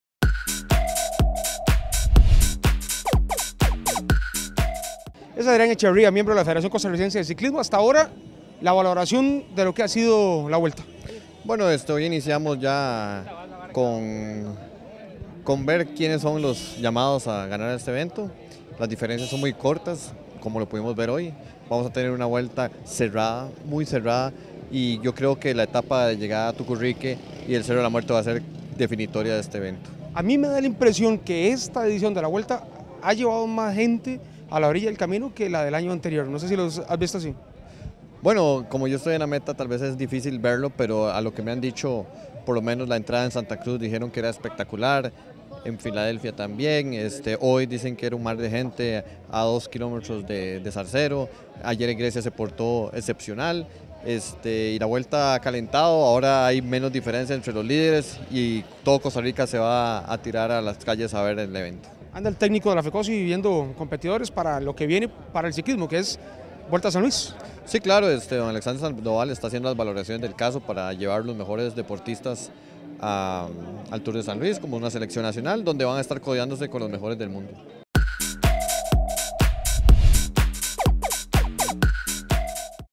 Vuelta CR 2015 - 5ta Etapa: Adrián Echeverría
Cómo miembro de la Fecoci, nos da su análisis de las primeras 5 etapas de la Vuelta a Costa Rica.